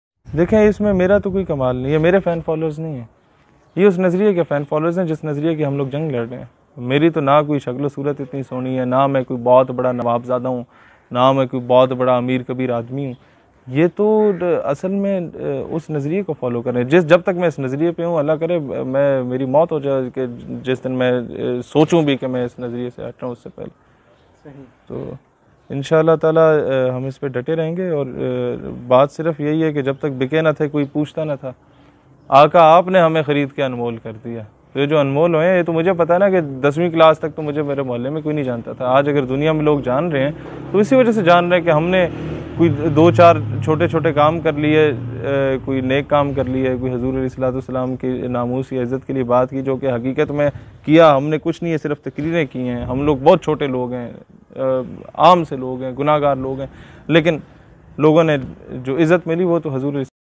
How many Followers does || Shaheer Sialvi Official
Shaheer Sialvi: My message for youth is as in the words of Jon Elia;
“aao k ikhtilaf-e-raye pr itefaq kar len”
People die voluntarily for Kashmir with only one wish, that is to be buried in Pakistan, wrapped in the Pakistani flag. This is the degree of their passion. They are not martyred for asking for freedom but are punished for lifting the Pakistani flag, they are punished for wanting to be buried in the Pakistani flag, they are punished for chanting the slogan “Pakistan Zindabad”.
For Kashmir, we need to push behind our political and sectarian differences and struggle for Pakistan. We must make Pakistan an Islamic welfare state. We must make it a role model. As Pakistan army is responsible to defend its geographic boundaries, so is our youth responsible to defend the ideological bounders of Pakistan. I would not only advise the youth but request and plead to join me for this cause.